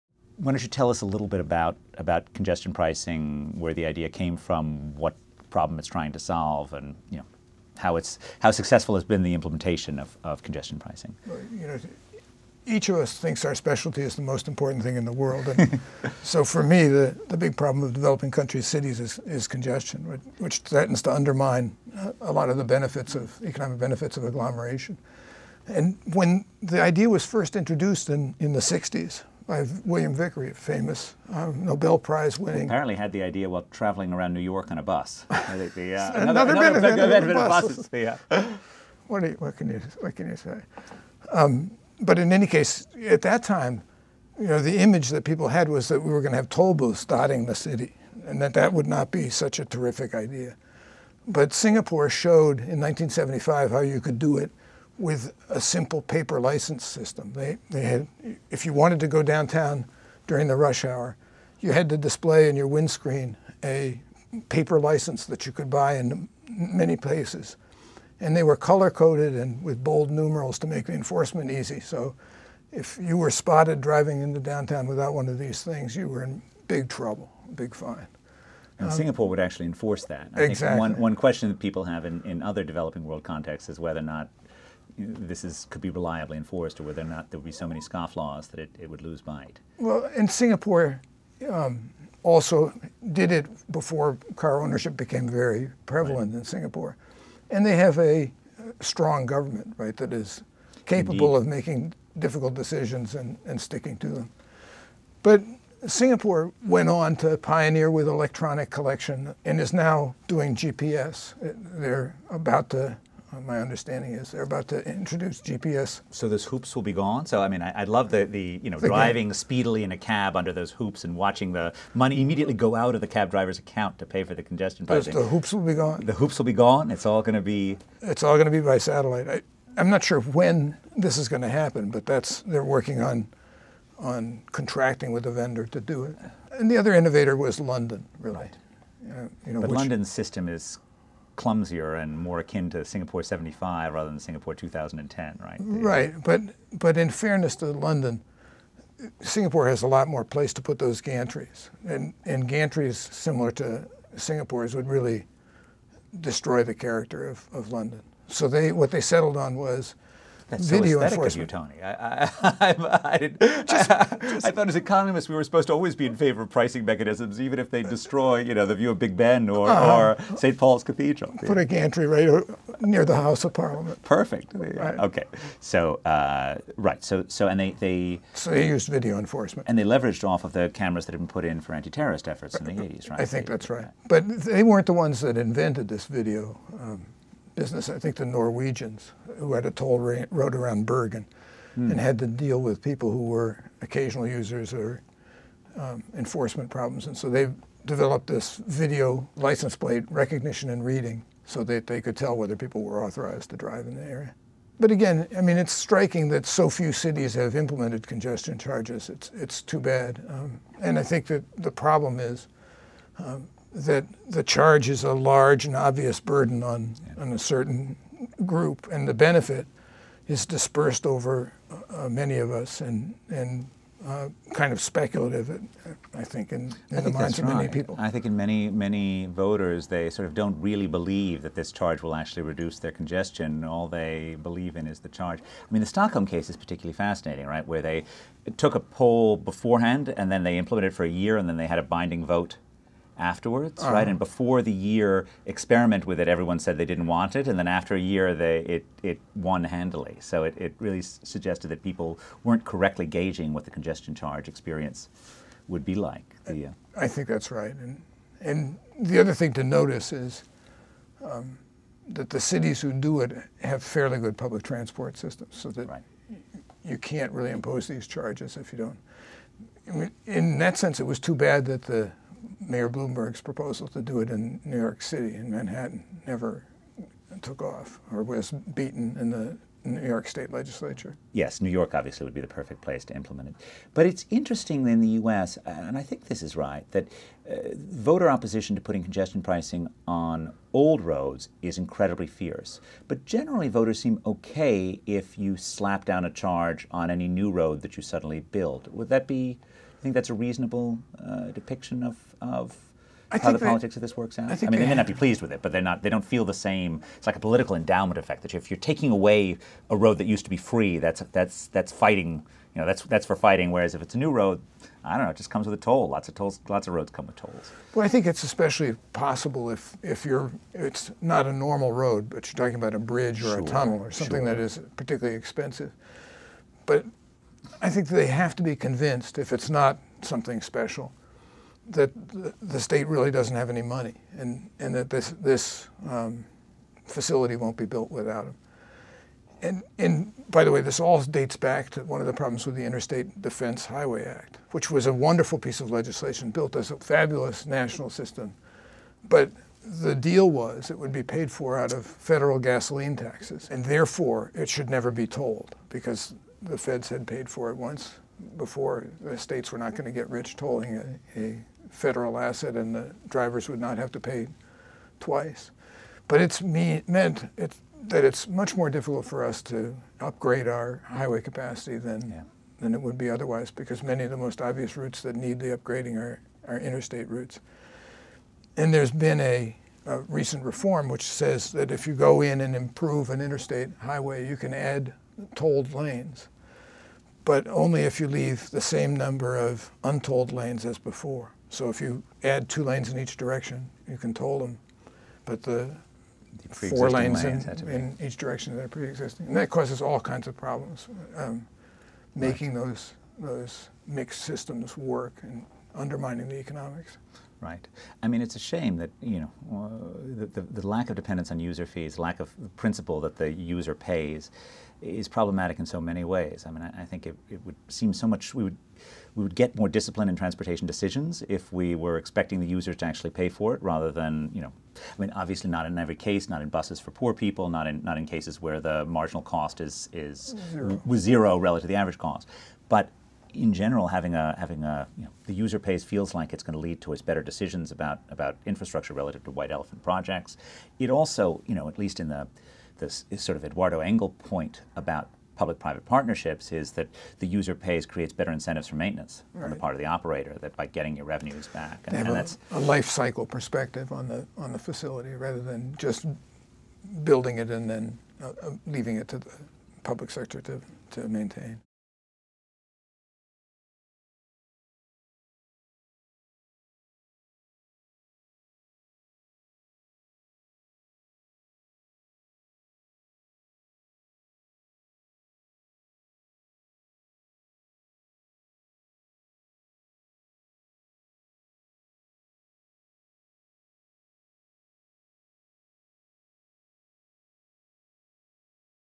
A Brief History of Congestion Pricing
Professor Tony Gomez-Ibanez explains why and how congestion pricing was introduced in Singapore, and discusses with Professor Glaeser why congestion pricing has succeeded in some cities, and not others.